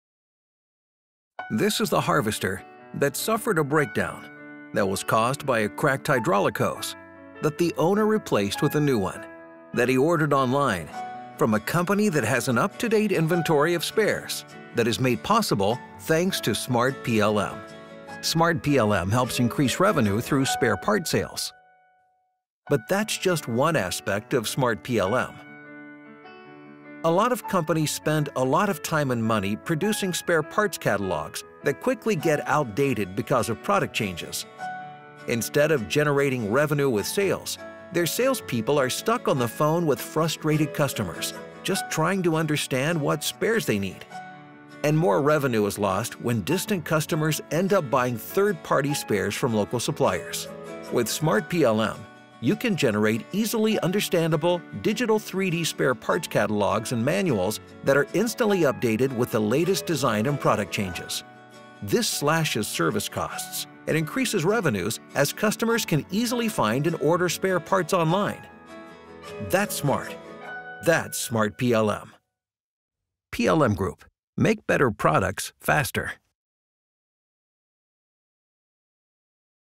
Smart PLM for people working in After Sales
In many companies, After Sales has a significant impact on revenue generation – directly with spare parts and services, and indirectly affecting rebuy from customers. However, it is a costly function with staff helping customers and stocked spare parts.

With Smart PLM, you can:
- Enable customers to find information and order spare parts online
- Secure easy access for staff to the latest and historical product versions
- Produce spare parts on demand using Additive Manufacturing
- Produce easily understandable manuals and instructions

This will enable your company to enjoy improved revenue growth – both top and bottom line.